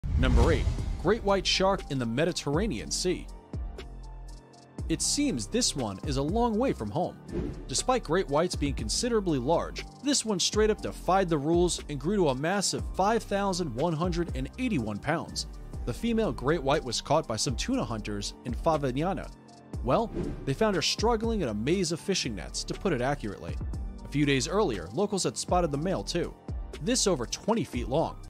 Great White Shark Caught in the Mediterranean Sea
After the movie Jaws, many people were too afraid to go into the water… and we don't blame them! Sharks are the top predators of the underwater world and evoke a feeling of dread and horror wherever they go. We don't have to tell you the sheer damage a shark is capable of if it gets its teeth into you, and in these 15 instances, it comes pretty close! Hold on to your chairs, and maybe put your feet up in case at any point you're afraid something will nip at your ankles. These are the 15 biggest sharks ever recorded on camera!